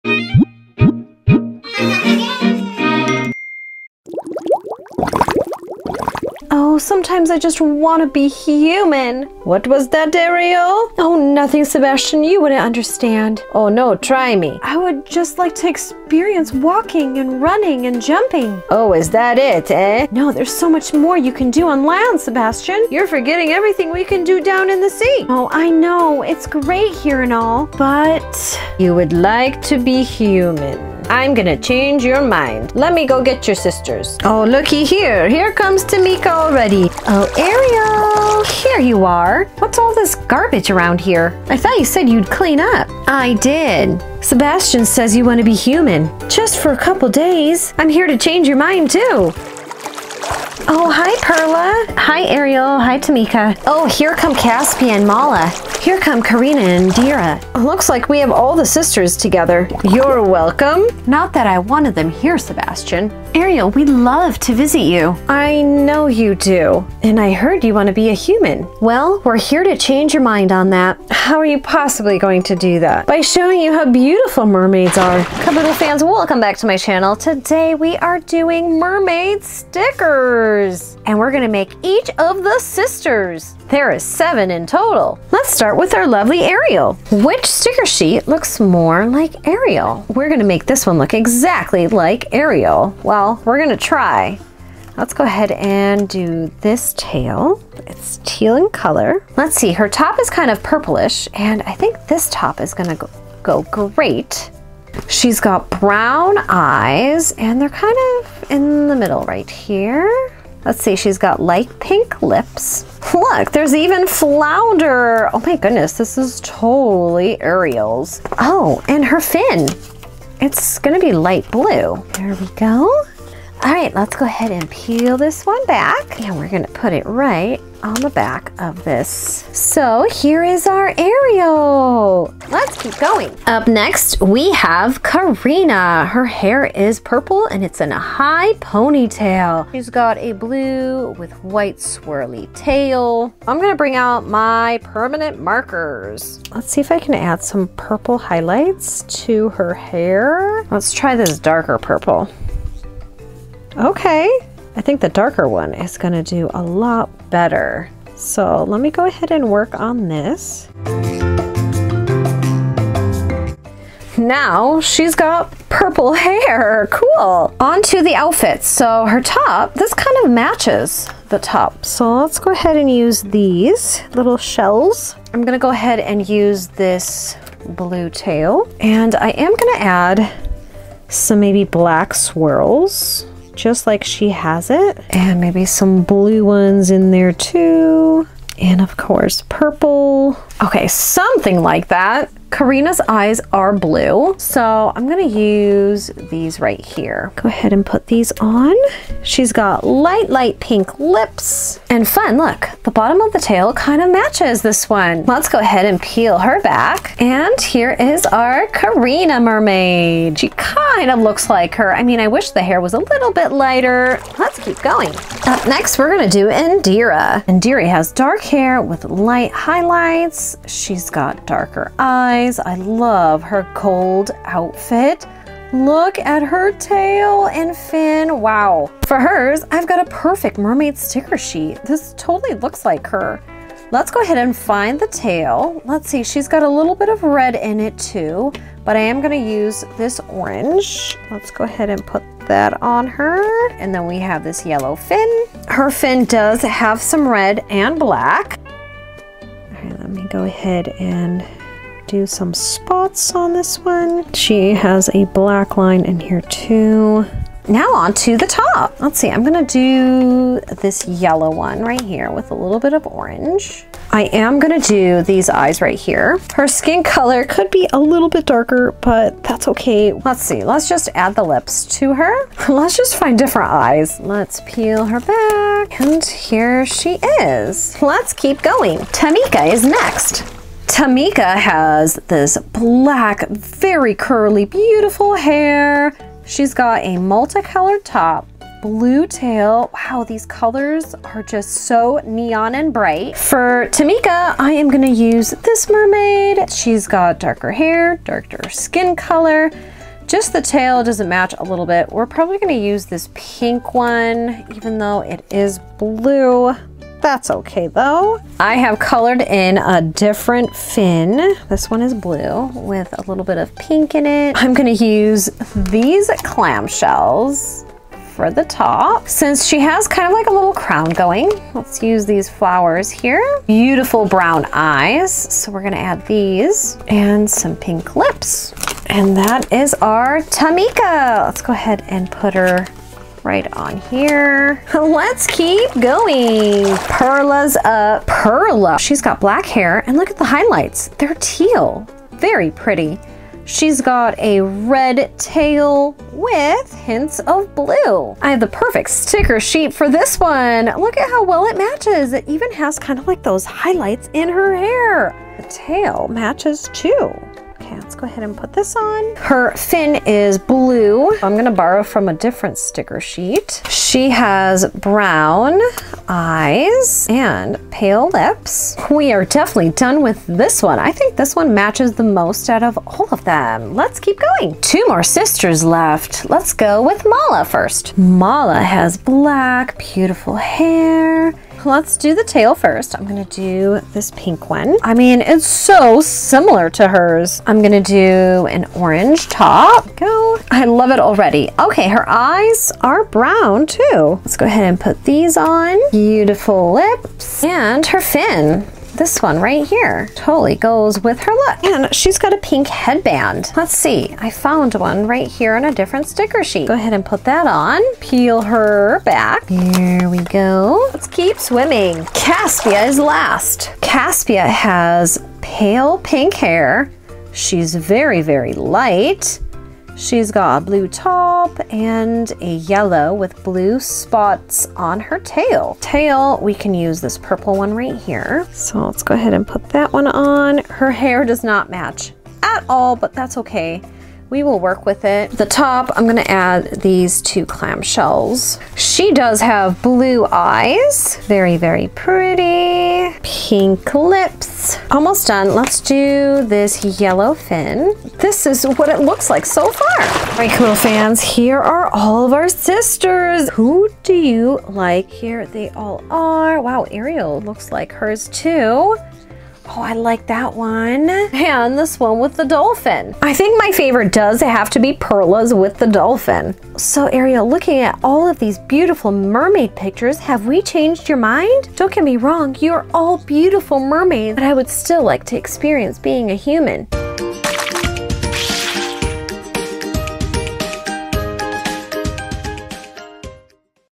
The Little Mermaid Movie 2023 DIY Make Your Own Face Stickers with Ariel and Sisters Dolls
Today we are making The Little Mermaid movie 2023 DIY Make your Own stickers with Ariel and all of the sisters. This kids craft includes sticker sheets of various characters. See what Little Mermaid pictures we decided to create.